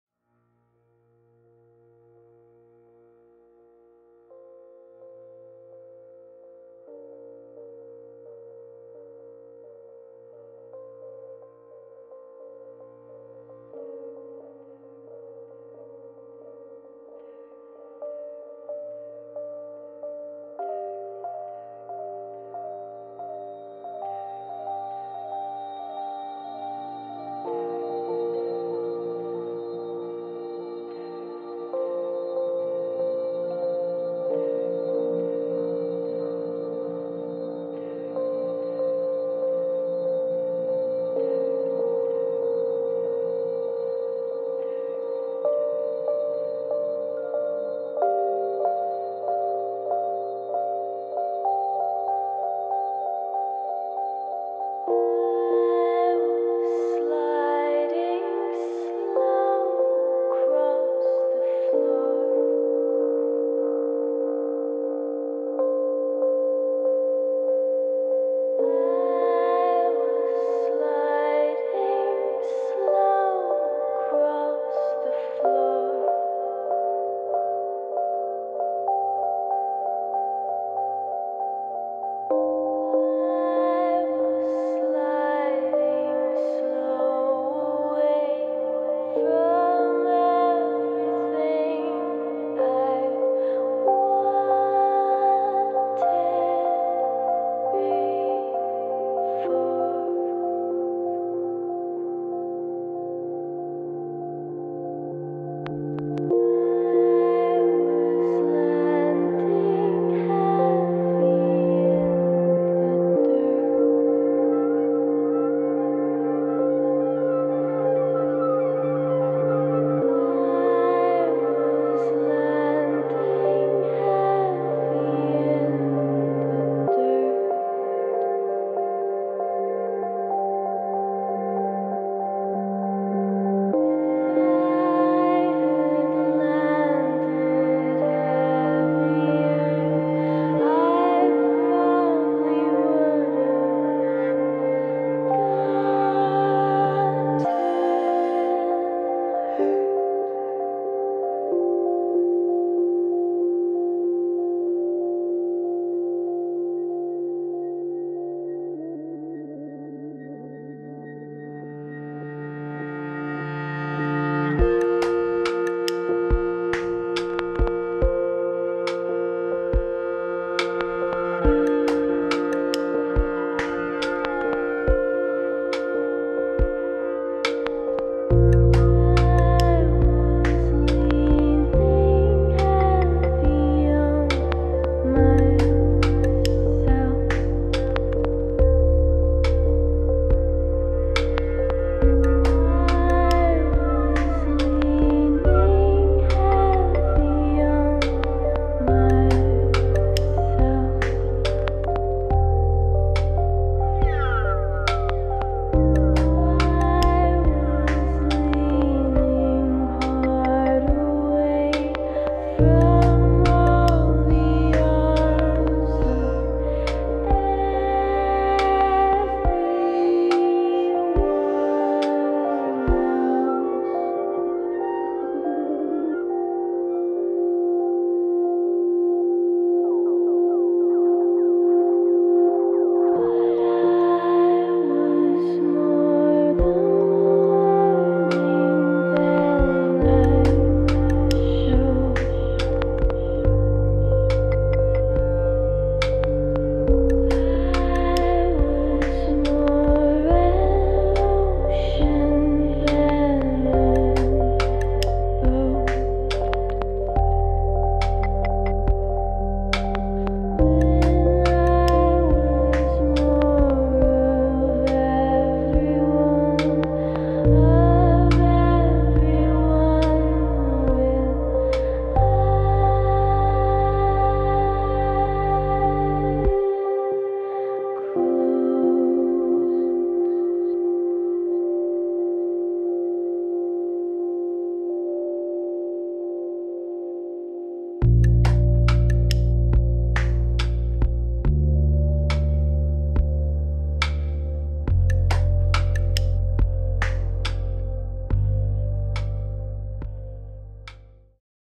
sliding through the spiral arm of the lissajous galaxy
radiator laser synthesizer and synesthesia video projection